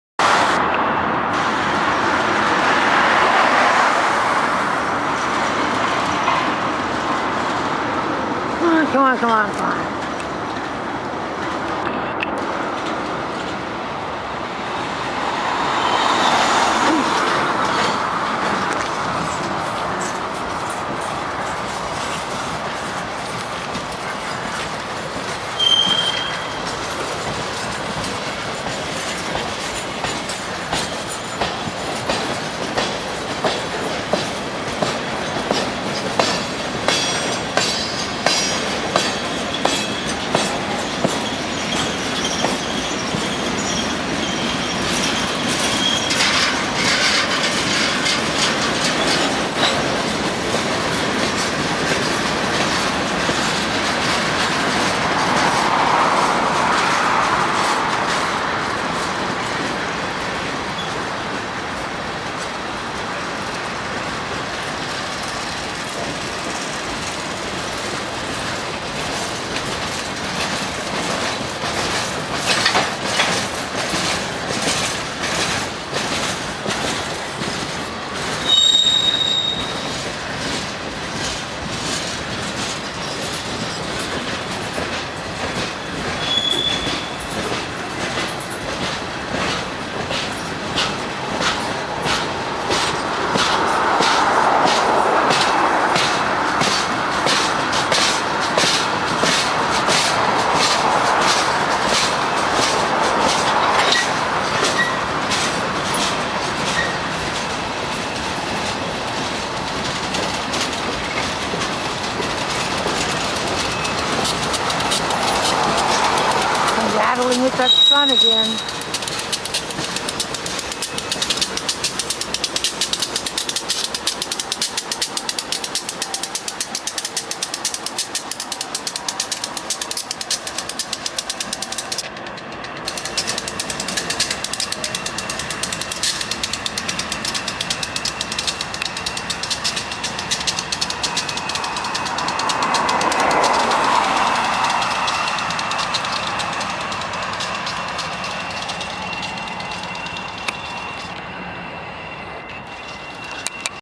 FREIGHT TRAIN GRAFFITI # 291
I live right next to the  tracks in Roseville, CA I am just learning digital photography & my camera is just a "point & shoot" Sony Cybershot so I  take videos for pure entertainment  The graffiti art I'm still learning to read so any help in identifying the artists will be greatly appreciated, can only read the simple ones! THANX  (P.S. the same goes with my photography skills! lol) Please "Like" video if you do so I have an idea what you want to see!